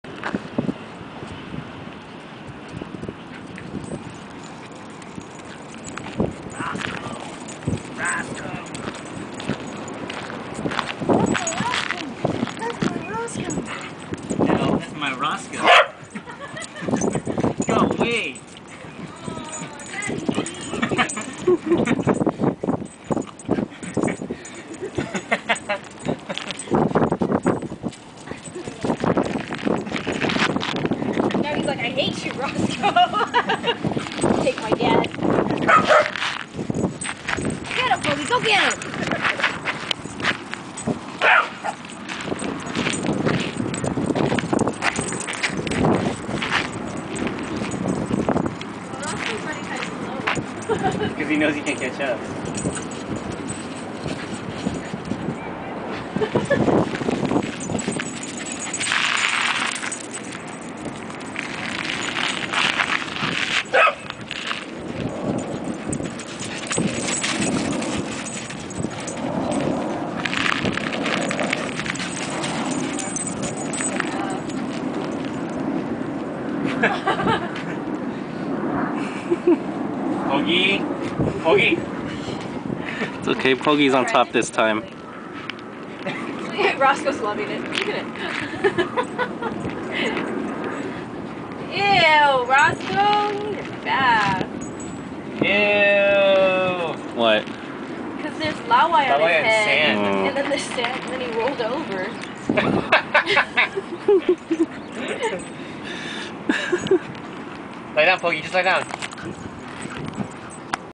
Pogi Chases Roscoe
Pogi - Welsh Corgi
Roscoe - Pomeranian Terrier